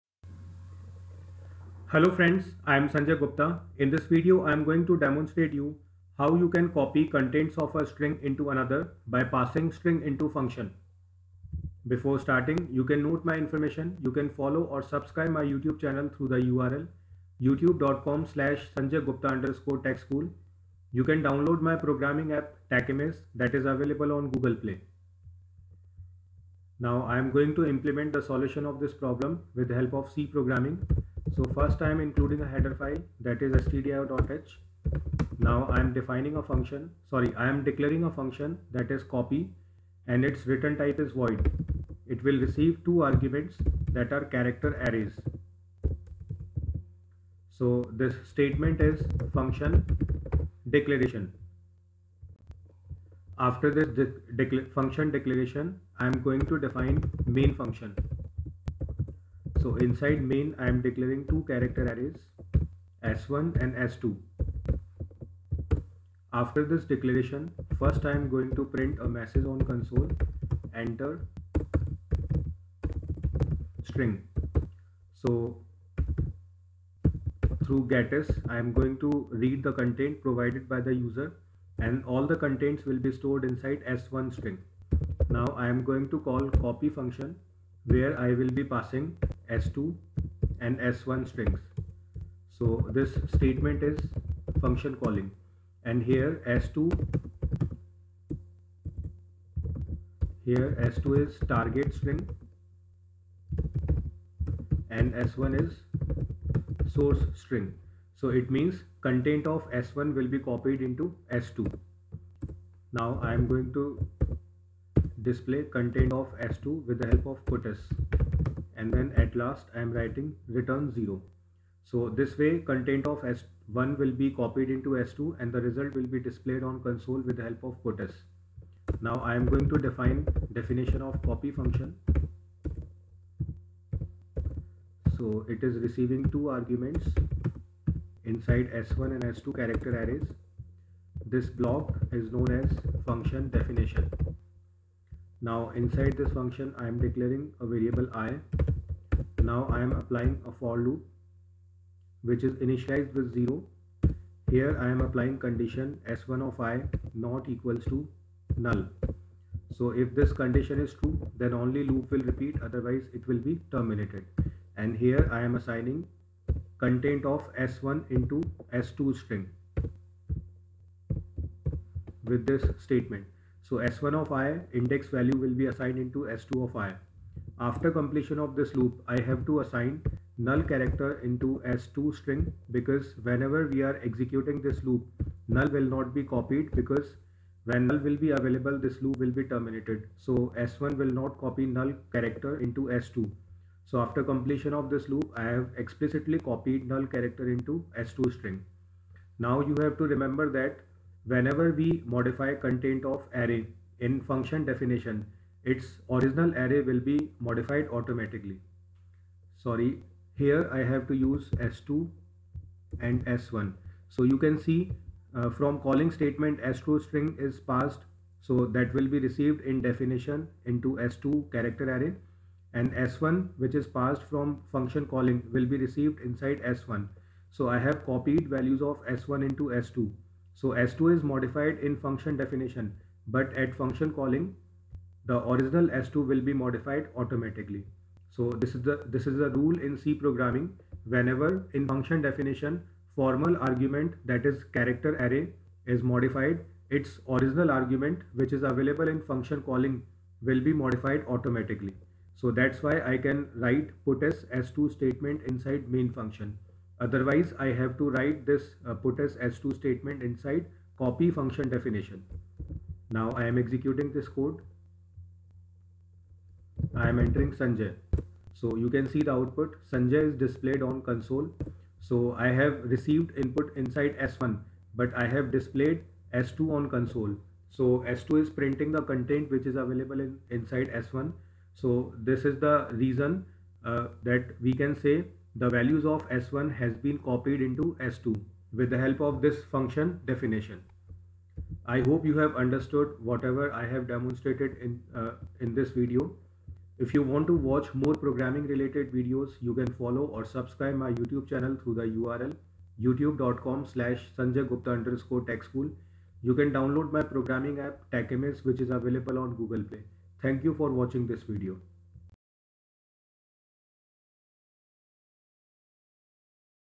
Copy a string into another by passing string into function in c programming | by Sanjay Gupta
How to copy a string into another by passing string into function in c programming language by sanjay gupta.